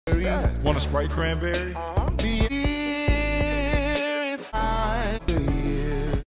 the sprite winter spiced cranberry commercial but only when lebron is on screen
ok i'm gonna stop with the sprite cranberry memes

for now